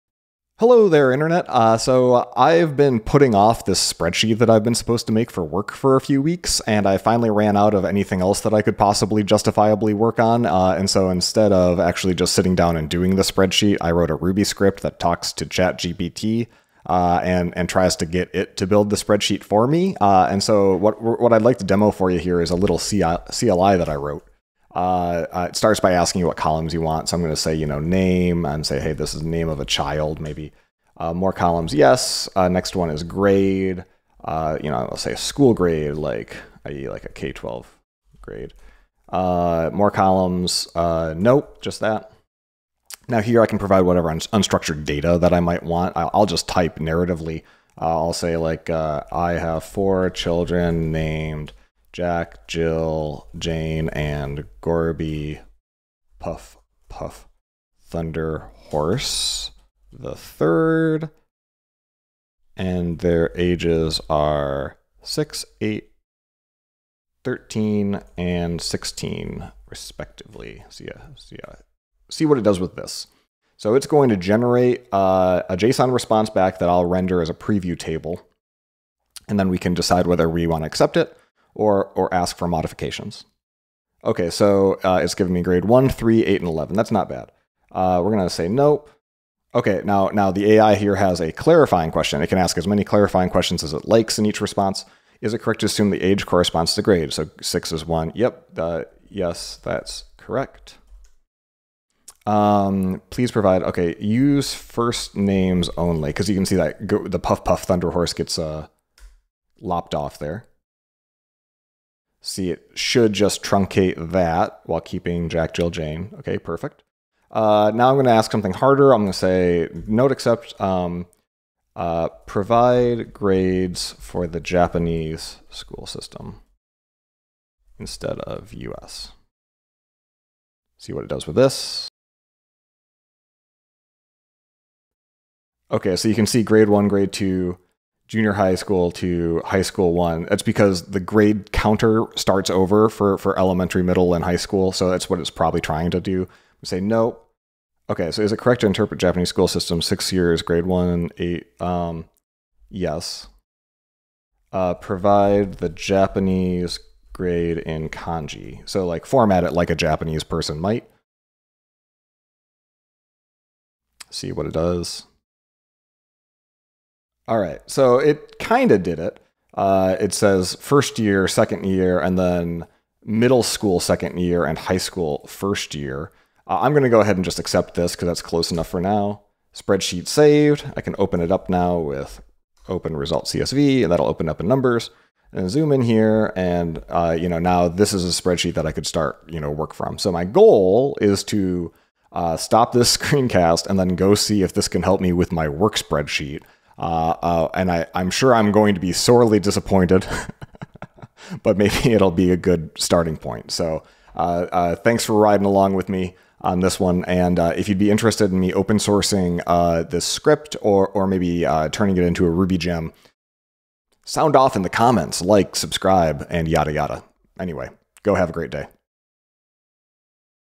Make ChatGPT build your spreadsheets for you!
I have a bunch of unstructured data that I'm supposed to organize into a spreadsheet and I'm really struggling to work through it all, so I wrote a Ruby CLI that uses OpenAI's GPT-4 API to specify the columns I want, paste in the data and instructions, and then interactively answer the AI's clarifying questions and provide follow-up requests until the spreadsheet is exactly how I want it.